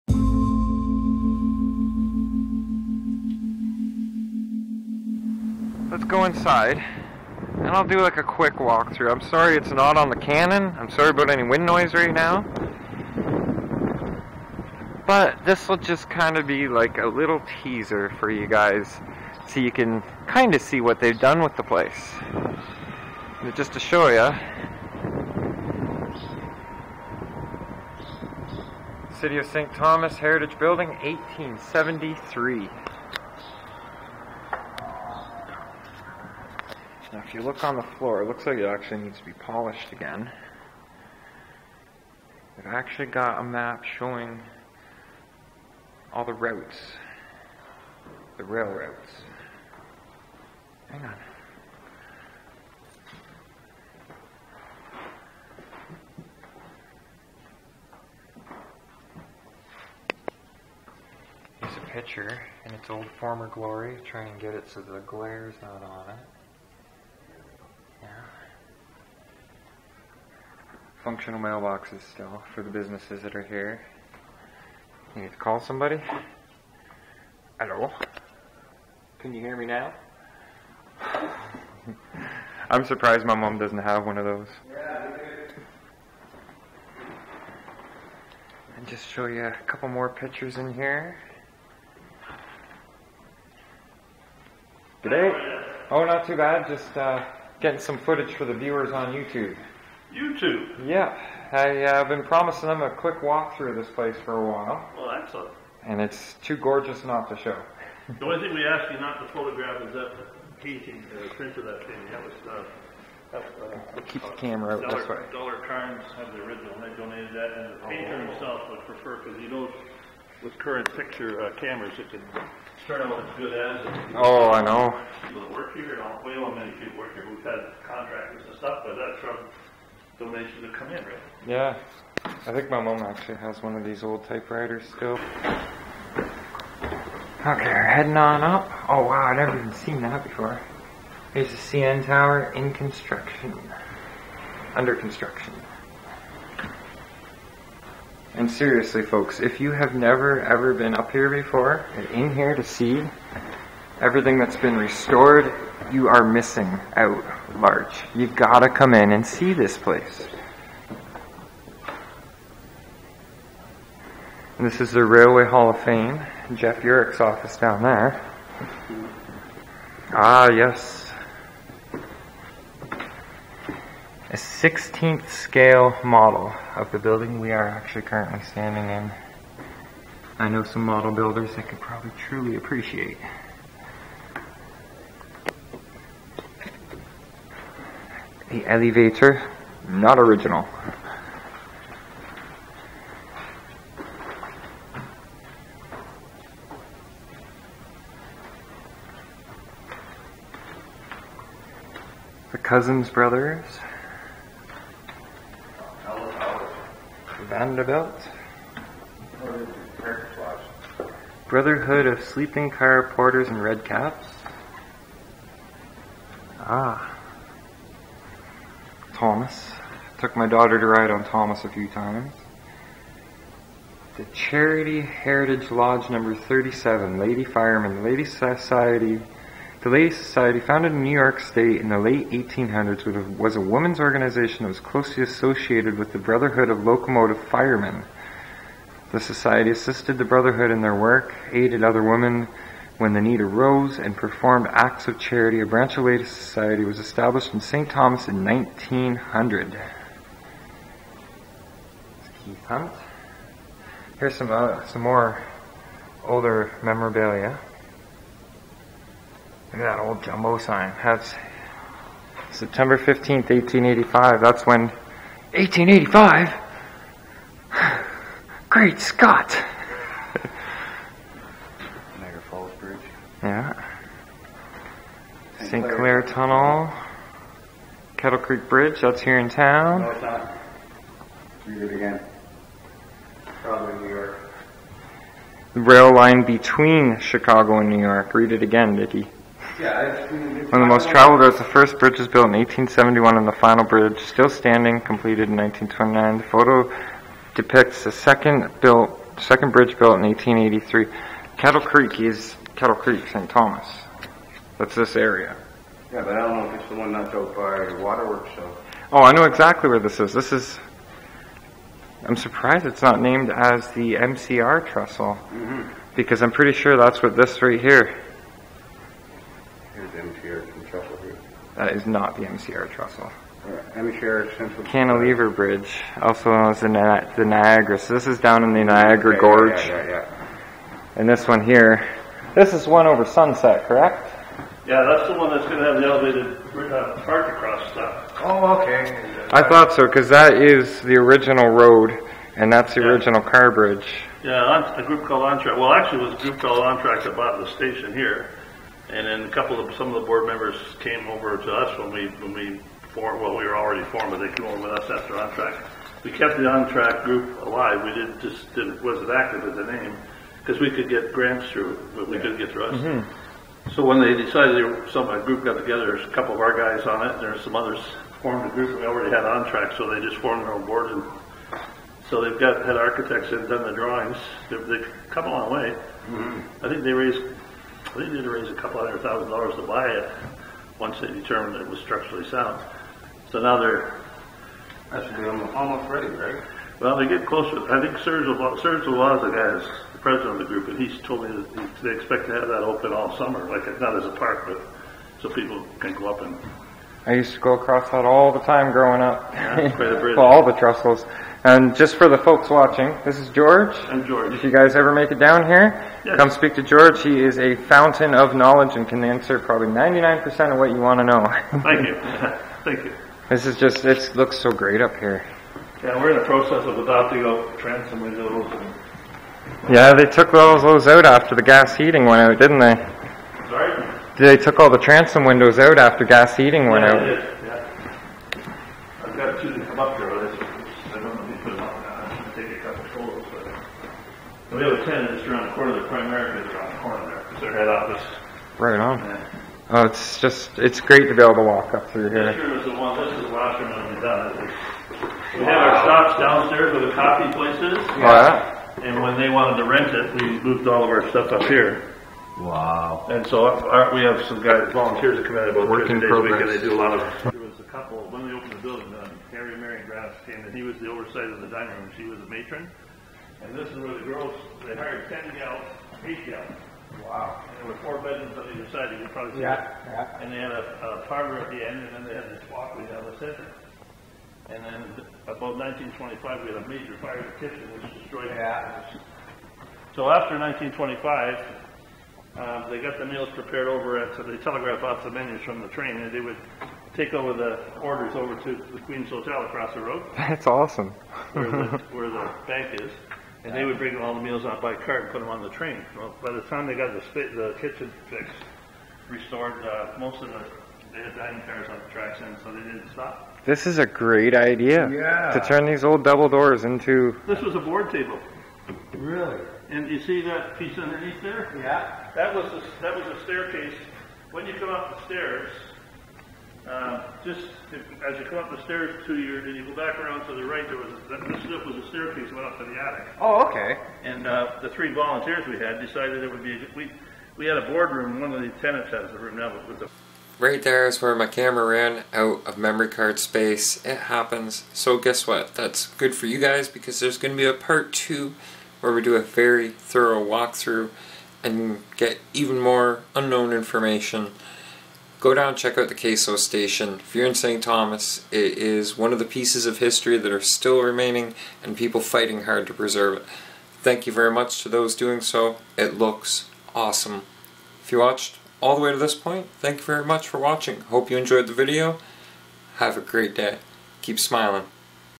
Canada Southern Railway Station (CASO Station) Walk through.
Here is a quick walk through of the Canada Southern Railway Station I recorded last week.. Please keep in mind this is only PART 2.